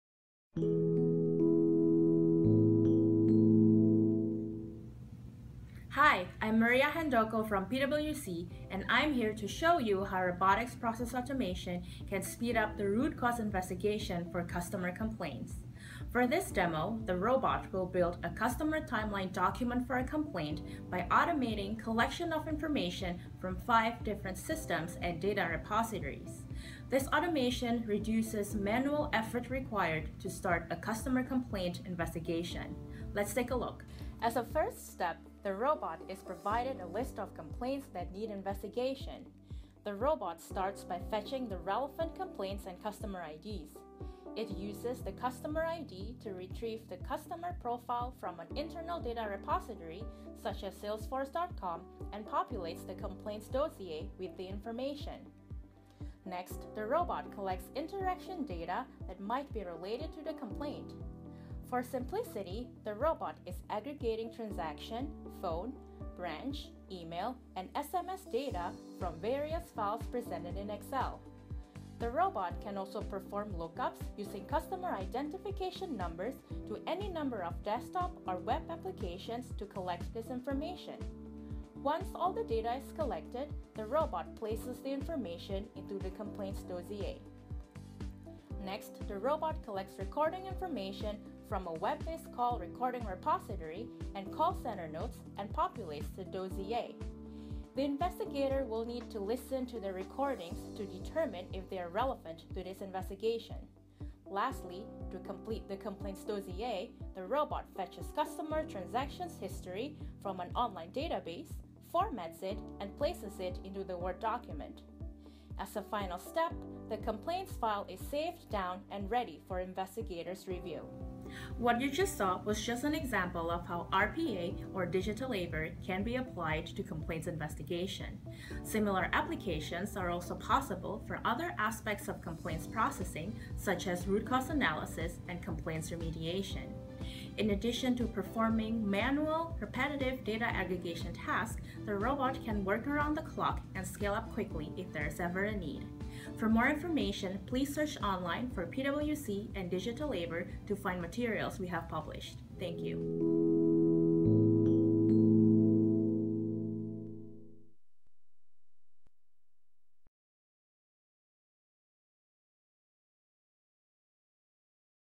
How can Robotics Process Automation speed up the root cause investigation for customers complaints?
PwC's FTO Senior Associate. Maria Handoko, walks us through an RPA practical application.